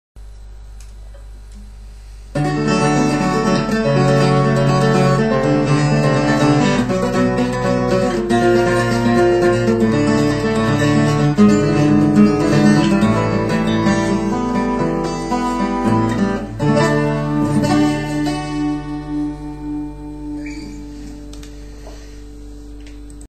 Fast, short and simple composition
A fast arpeggiated chord progression played on an unplugged 12-string guitar.